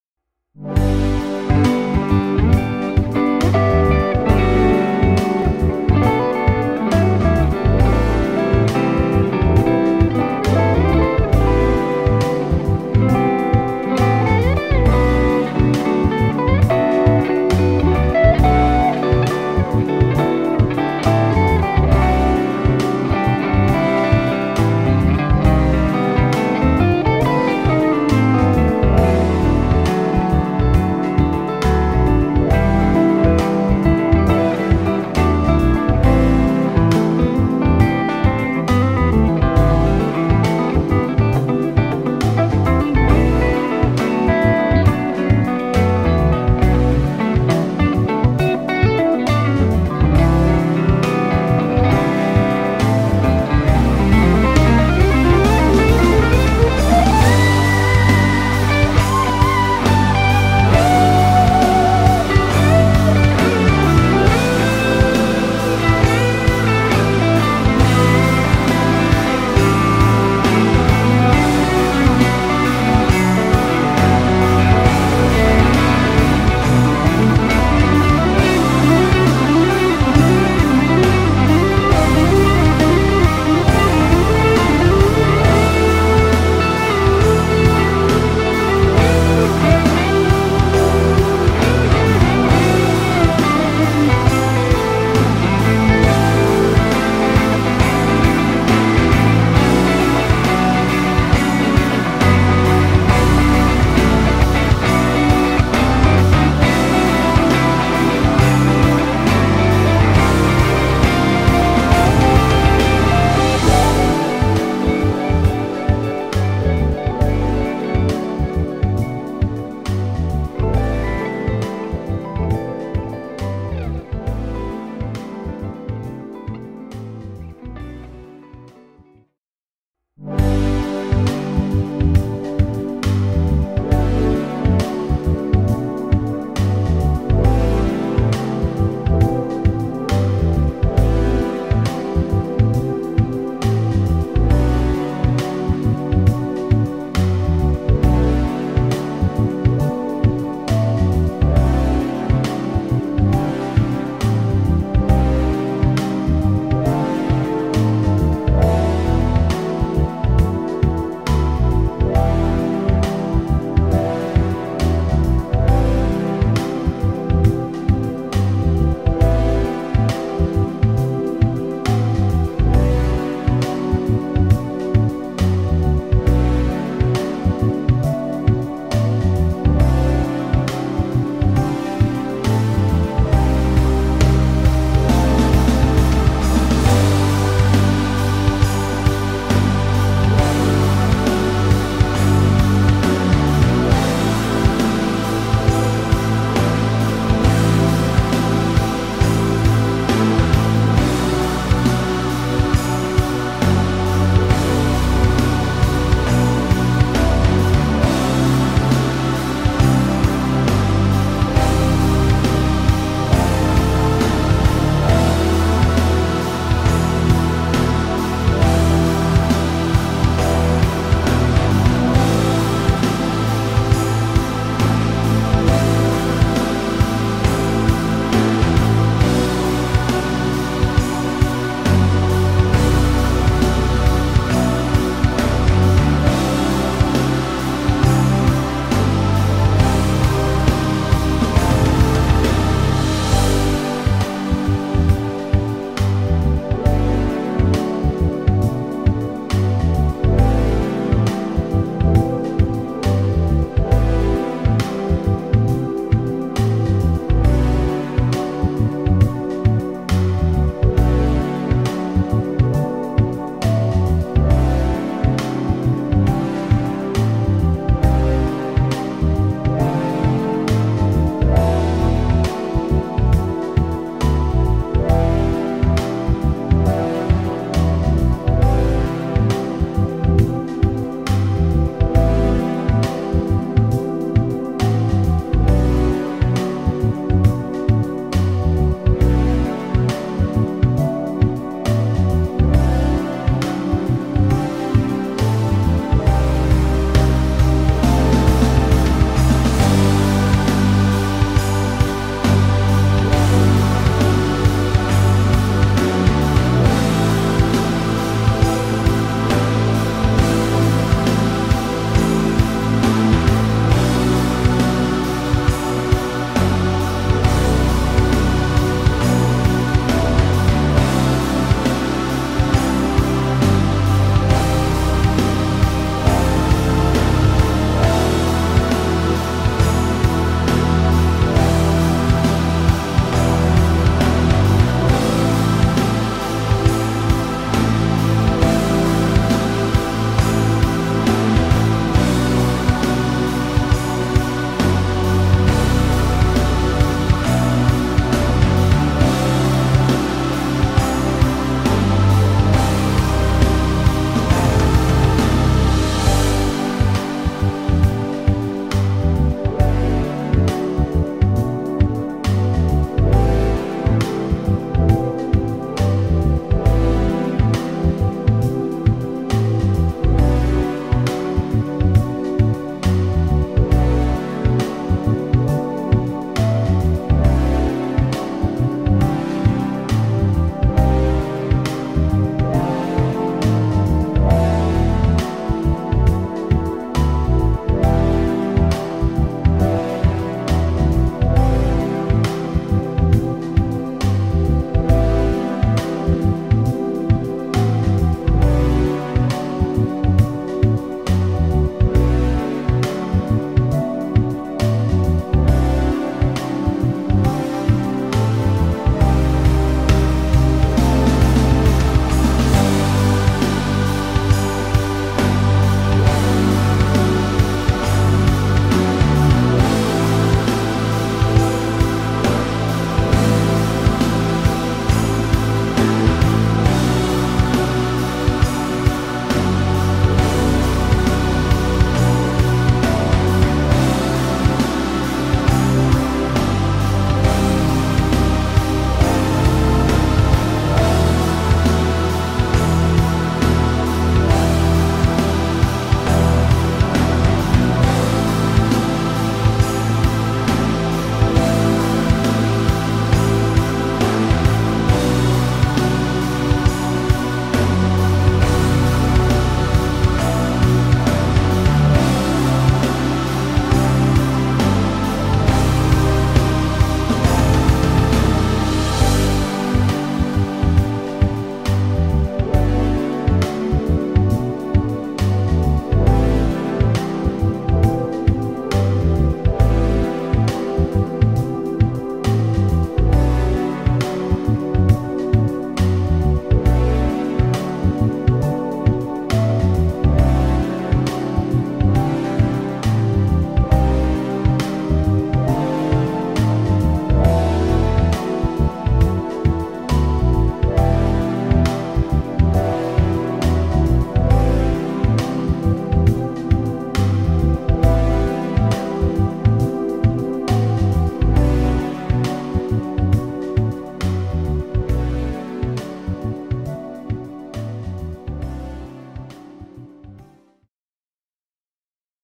Soft/Loud Backing Track | G major
Mp3's of all my tracks, and teaching you everything I know

This particular track is in G major, but contains a minor 4 chord, which in this case is C minor.

Solo (0:00)
Track (2:08)